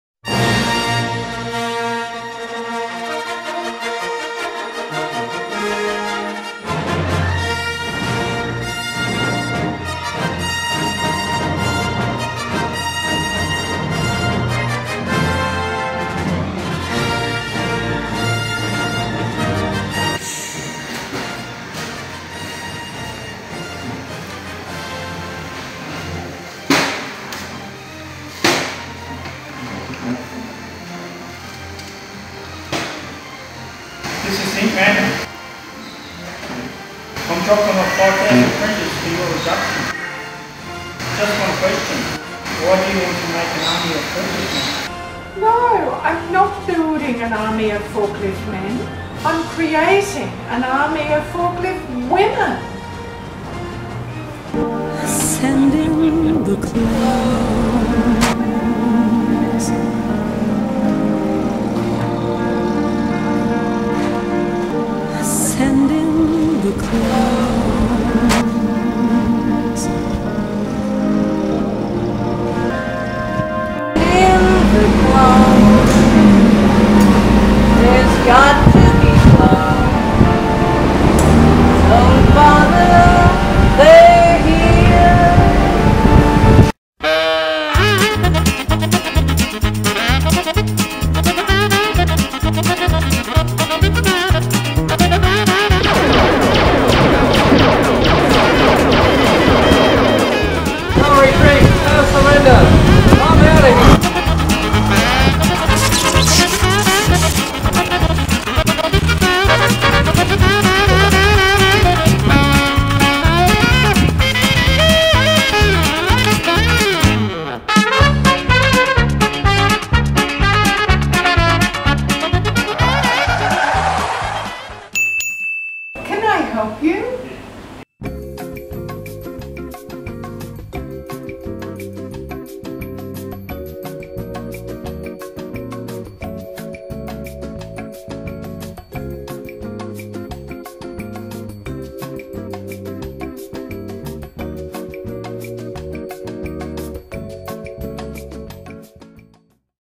Forklift Man - Clone Wars
An army of Forklift Men have been sent out to attack Forklift Man.